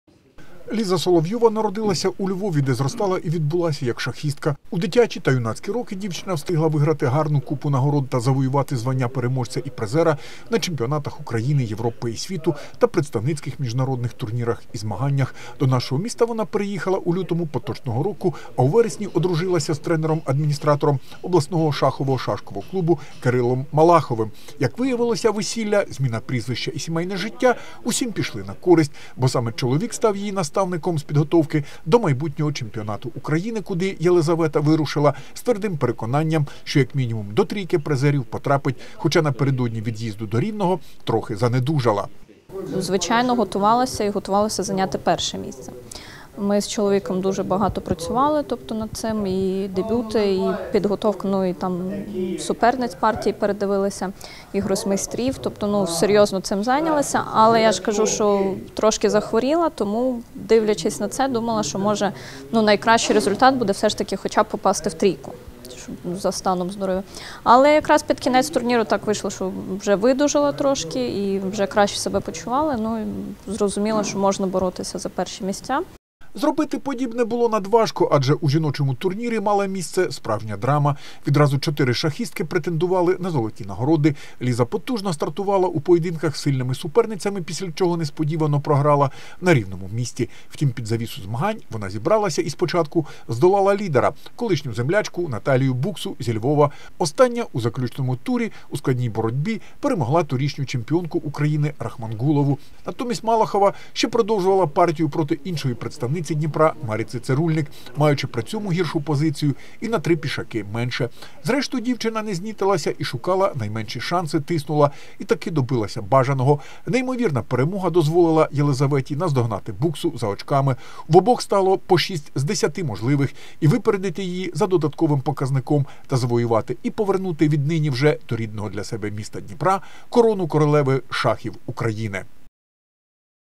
«Жіноча шахова корона чемпіонки країни знову повернулася до Дніпра»
У сімдесят третьому році минулого століття Олена Єлєніна стала чемпіонкою Радянського Союзу. Нині золоту нагороду чемпіонки України завоювала Єлизавета Малахова, яка здобула її на змаганнях, які щойно фінішували у Рівному.